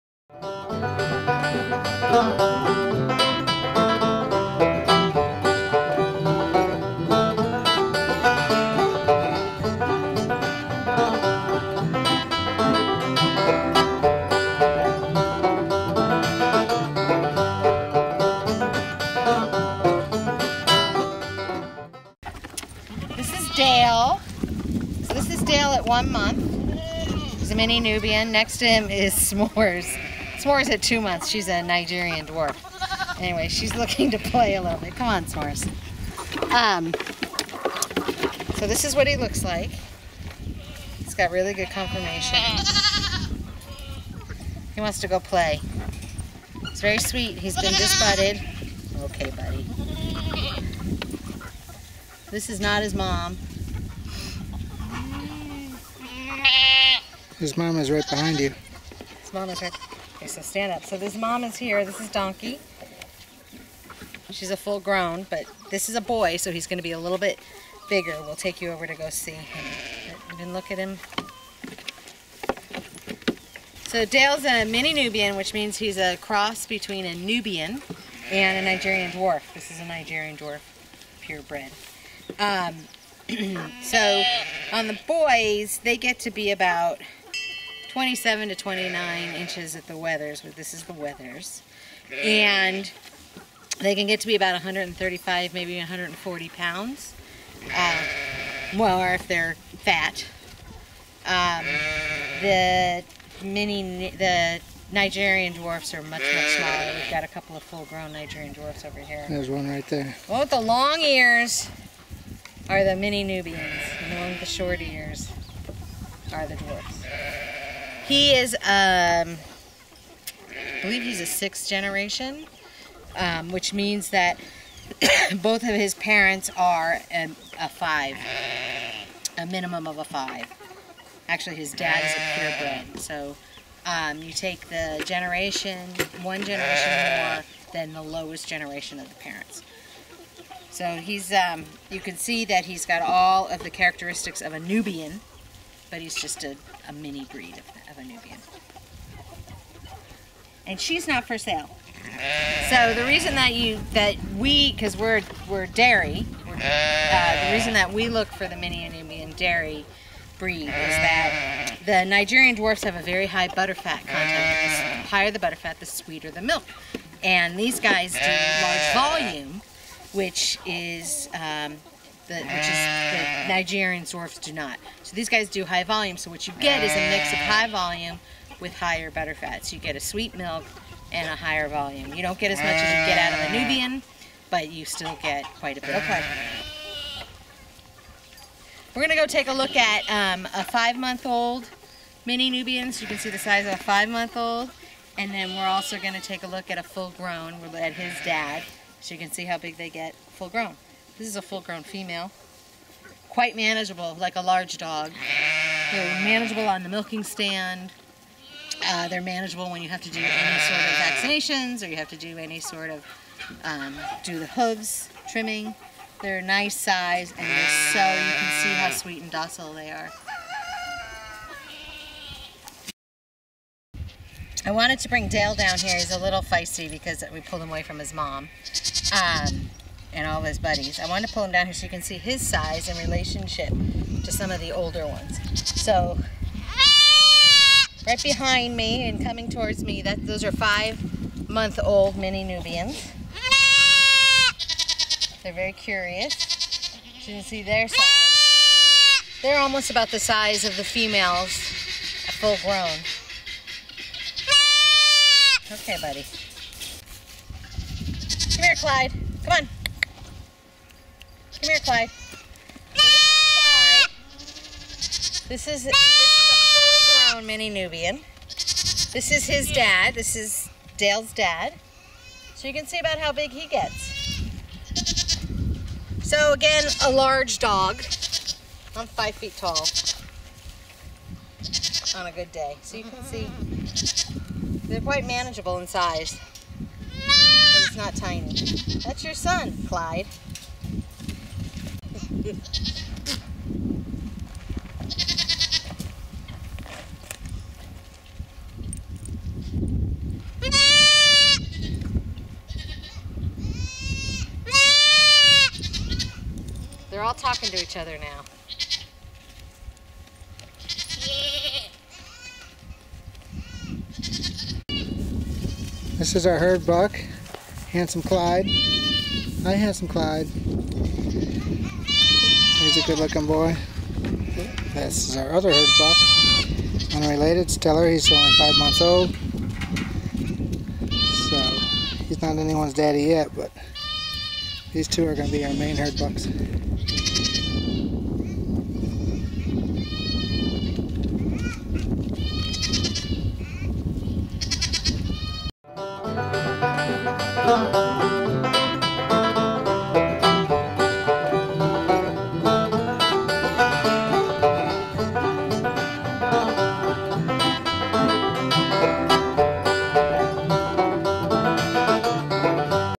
Mini Nubian Goats
Here we show and tell about our herd of mini nubian goats